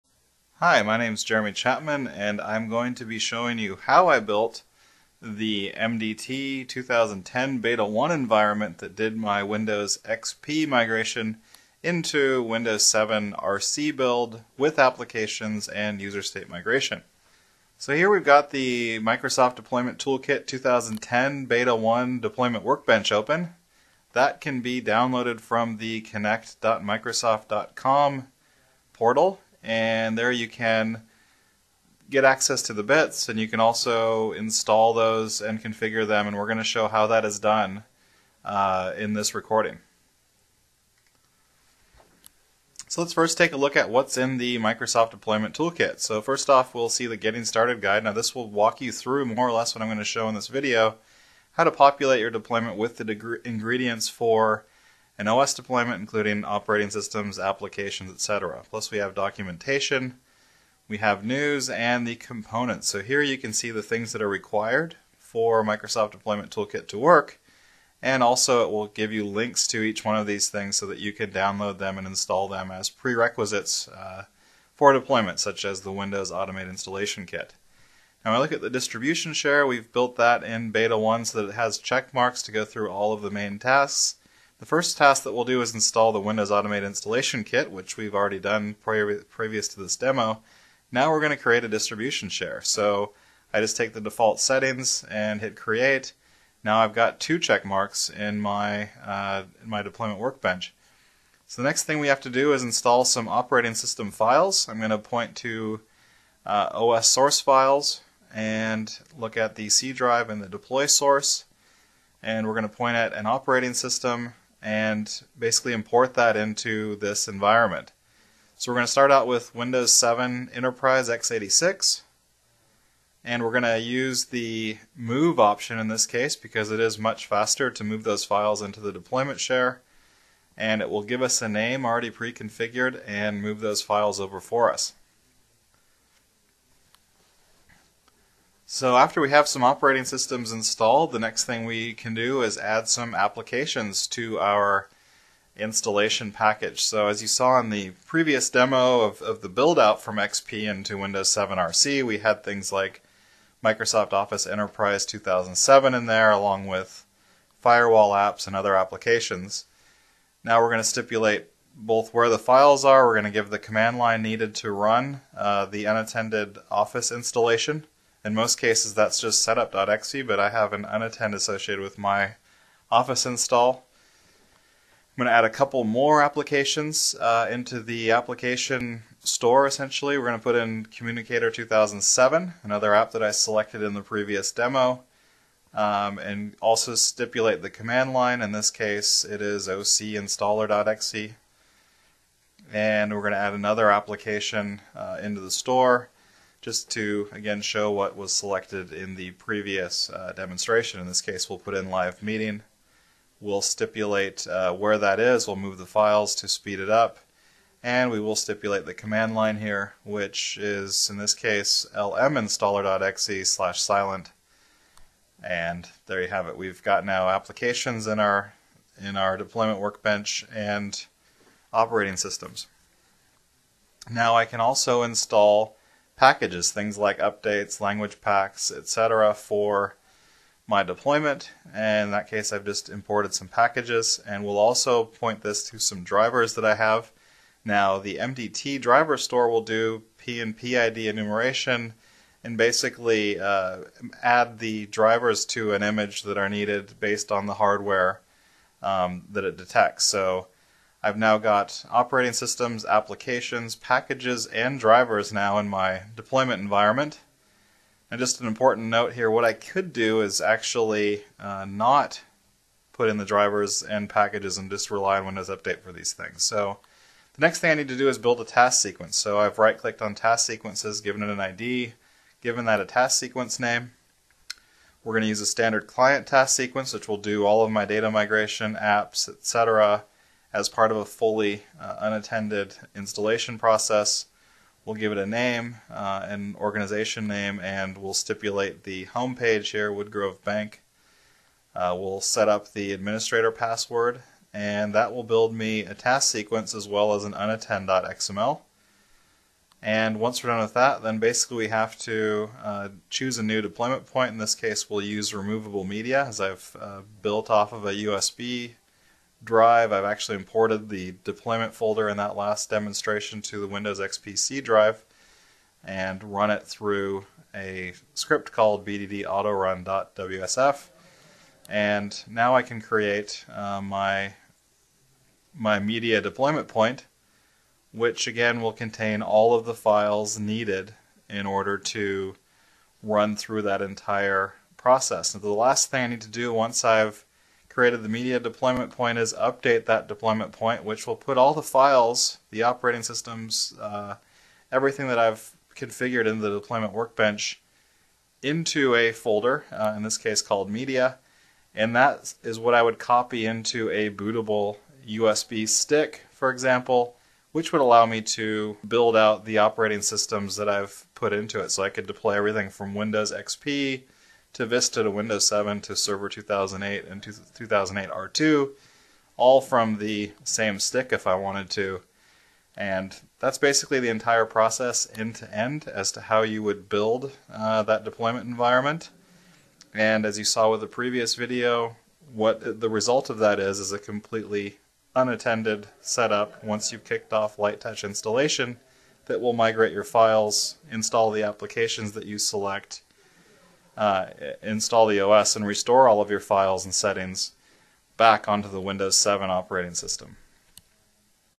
Windows XP Migration to Windows 7 RC using MDT 2010 Beta 1 Part 2: How To Build It
In part 2 of this 2-part series, Jeremy shows how he built out the migration environment using the Microsoft Deployment Toolkit 2010 Beta

1. After browsing the Deployment Workbench, Jeremy adds operating systems, applications, packages and drivers then creates a standard client task sequence to manage all the steps in the migration. Just about everything is wizard-based and the hardest part in of the installation as shown is finding the command lines used to install the imported applications. If you havent seen part 1 already, we encourage you to start there. For more information on the tools and processes, on TechNet.